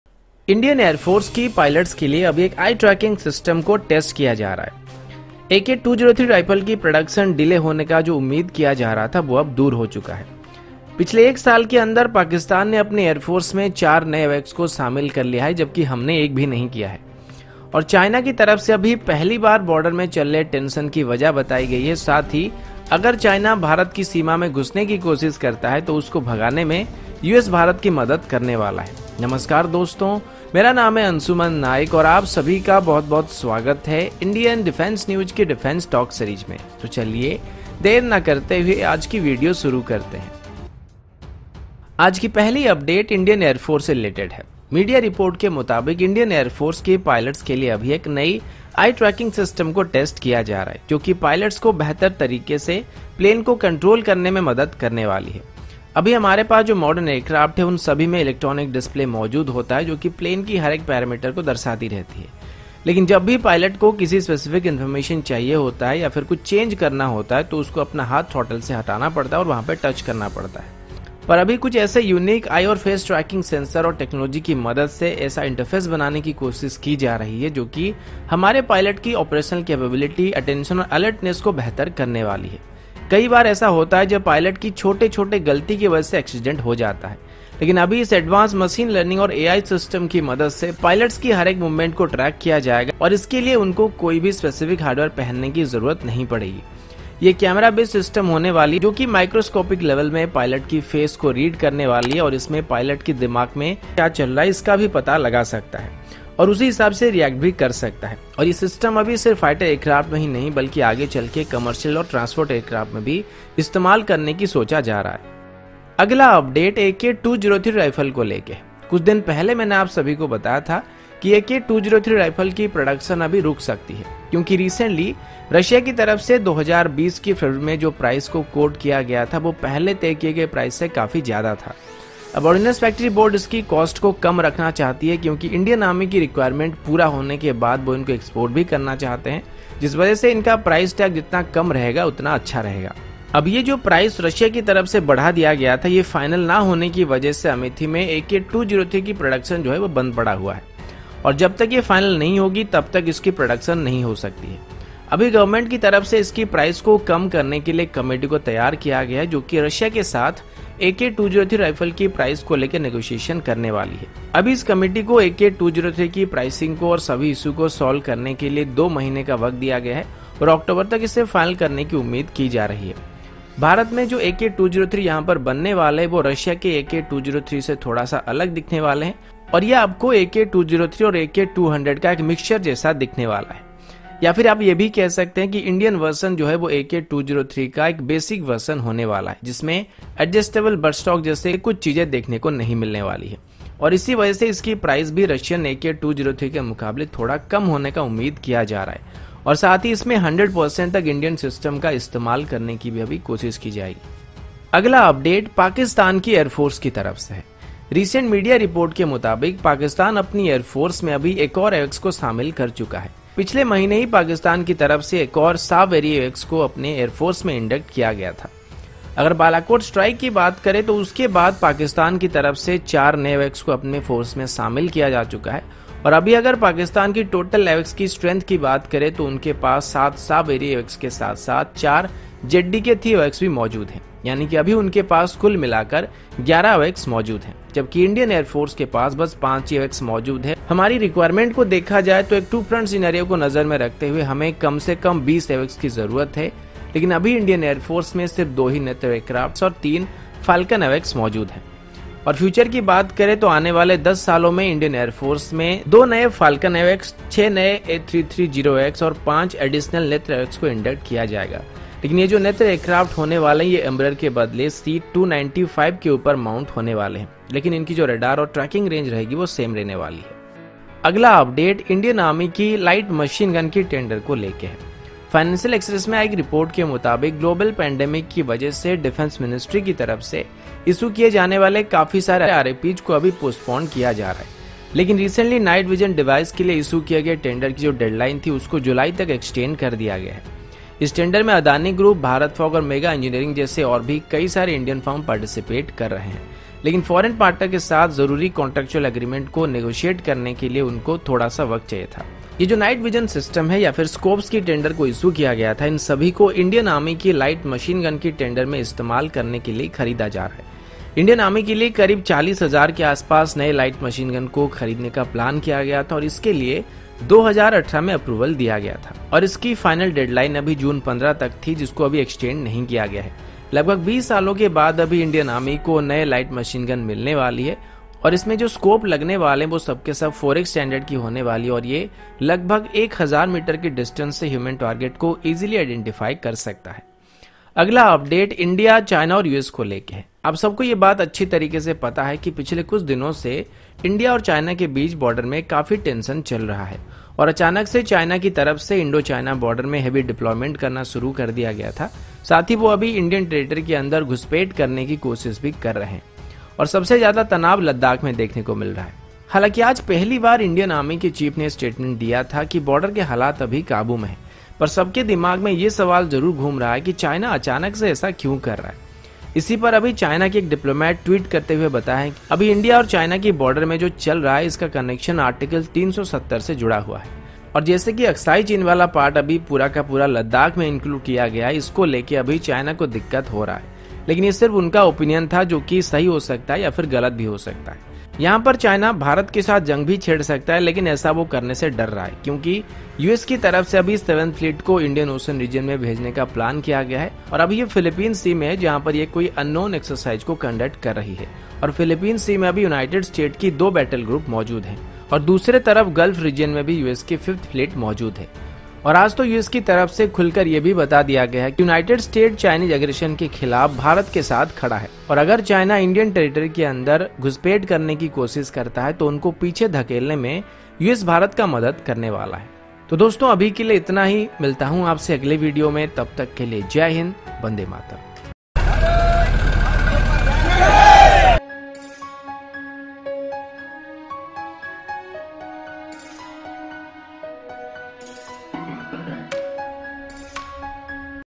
Indian Defence News:US will Support India if China attacks,PAF induct 4 AWACS in past year,Track-eye
Today's Latest Indian Defence News/Indian defence Updates in Hindi As Follow:Indian Defence News:US will Support India if China attacks,PAF induct 4 AWACS in past year,Track-eye

-Indian military tests eye-tracking tech to help pilots control planes
-Amethi rifle factory deadlock likely to be resolved, costing panel set up
-Jack of all trades, master of none: PAF gets 4th AWACS, IAF none since Balakot
-NO EXTENSION FOR BID SUBMISSION FOR 41,000 LMG PROGRAM
-US will always stand with India in pushing back against Chinese probing of Indian sovereignty: Top US diplomat